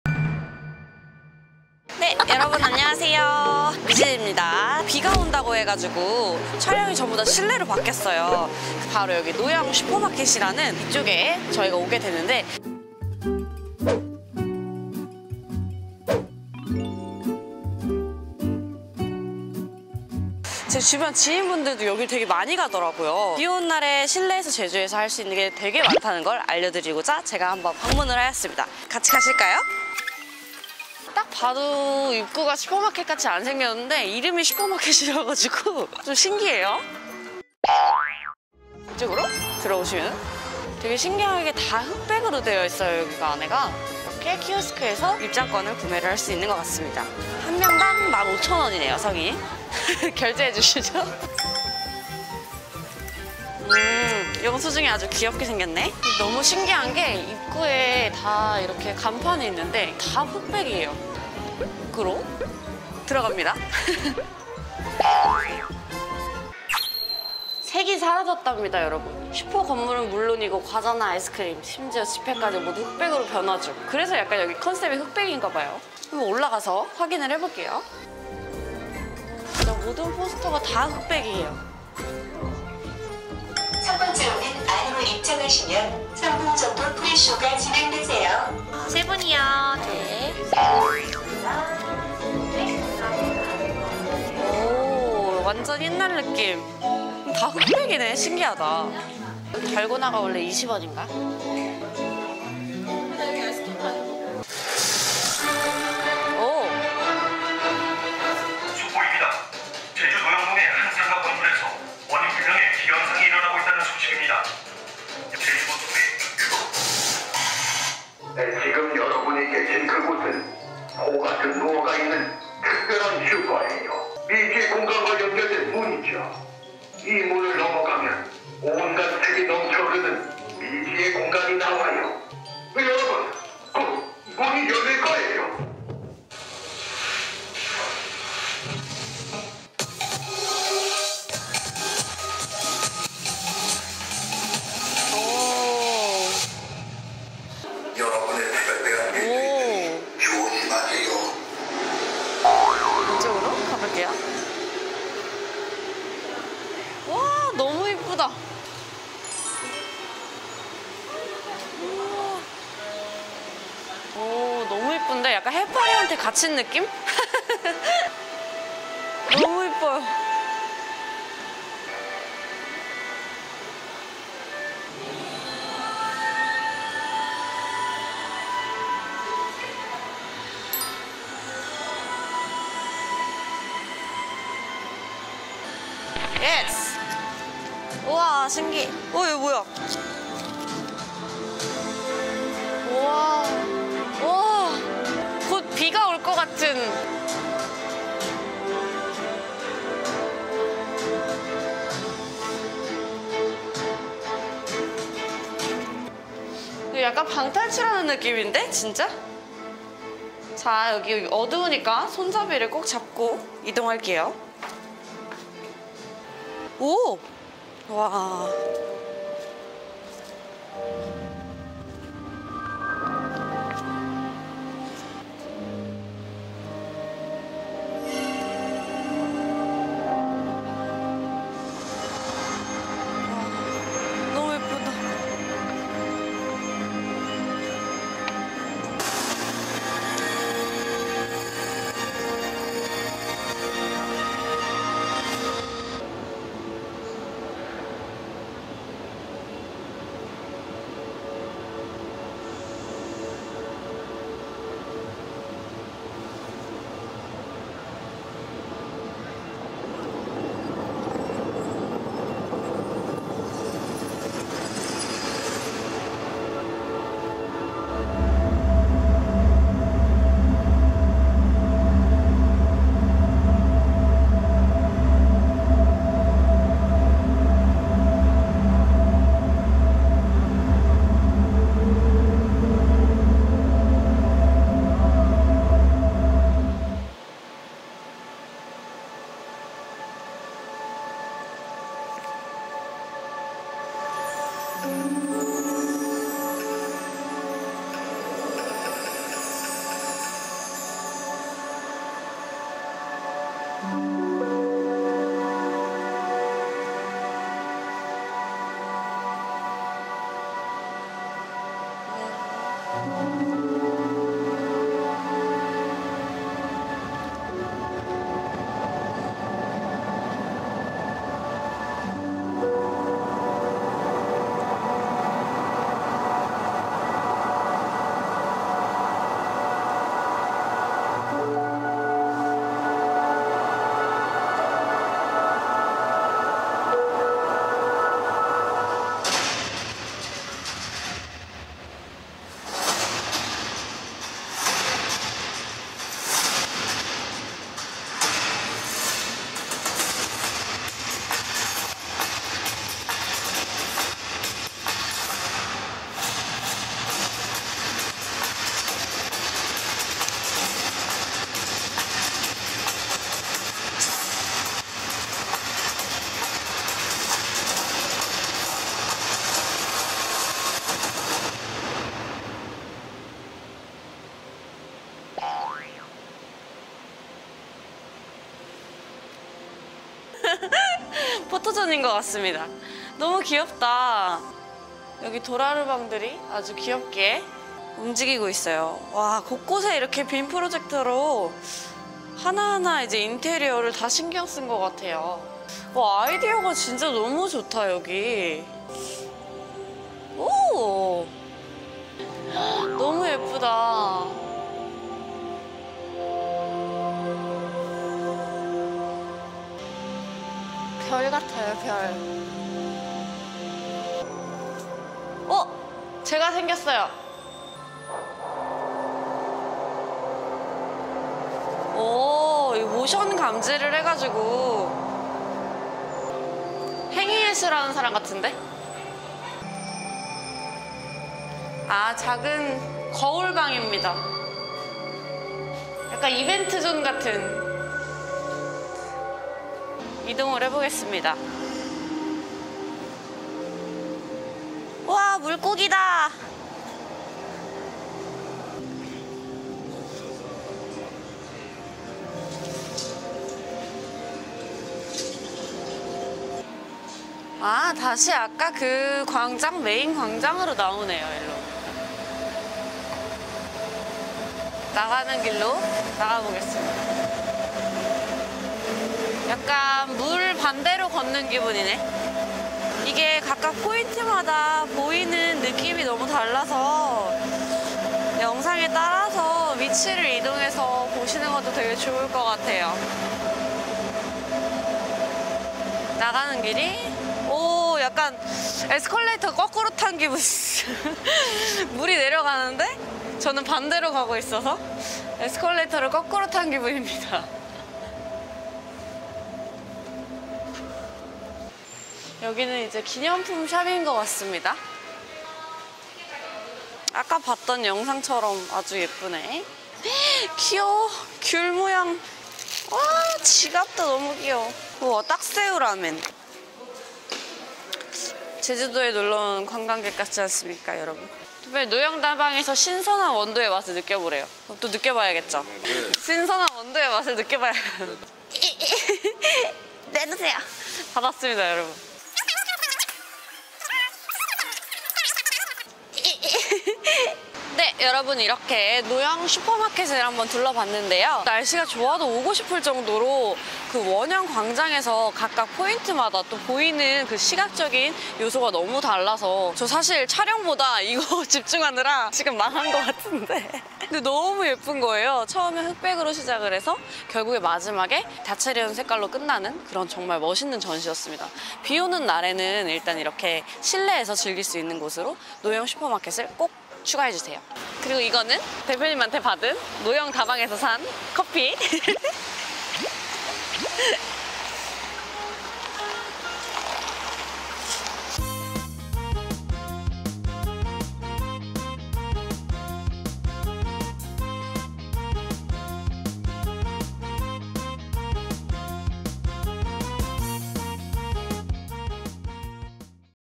제주도 비오는날 갈때 없으면 여기로 비오는날 가볼만한 노형슈퍼마켙 정보를 알려드립니다. - 크크바나나
제주도에 비가오면 갈때가 없을까요?
제주도 비올때 가볼만한 곳을 소개합니다.
구독🌞과 좋아요👍가 늘어날수록 크크바나나TV에겐 큰 힘이 됩니다♥